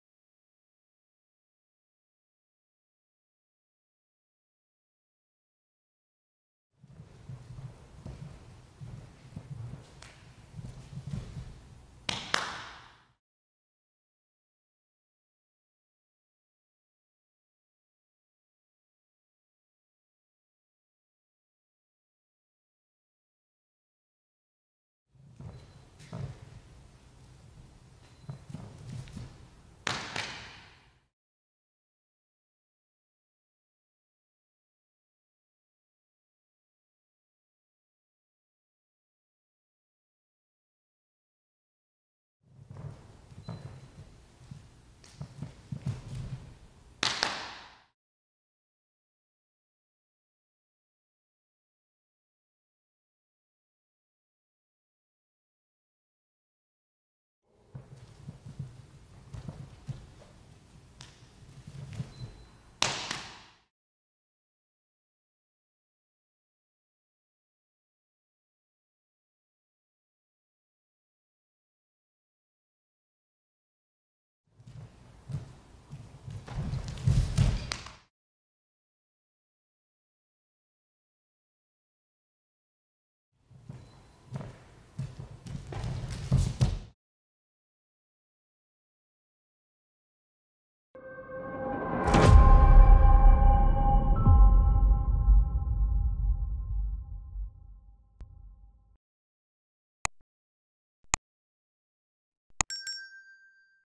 Shirt Grab Defense | Straight Elbow Lock
VB presents another classic self-defense technique present in GM Helio Gracie’s curriculum. This technique was “forgotten” for years until professor Pedro Valente found a manual showing this move and asked Helio Gracie about it.